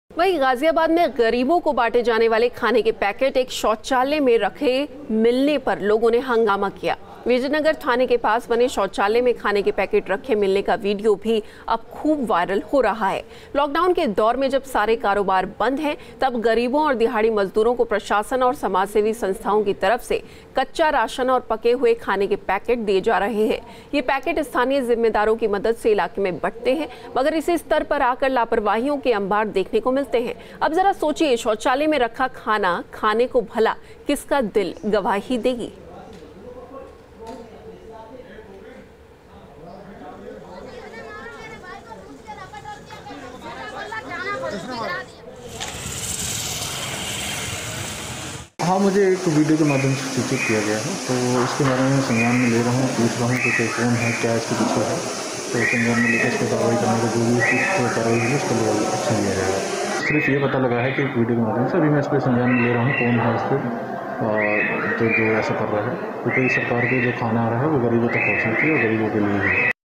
गाजियाबाद: लॉकडाउन की बदनुमा तस्वीर, शौचालय में खाने के पैकेट मिलने पर हंगामा
लॉकडाउन की बदनुमा तस्वीर, शौचालय में खाने के पैकेट मिलने पर हंगामा, देखें पूरे खबर.

News State Apps डाउनलोड करें:
Download Android and iOS Apps for more updates: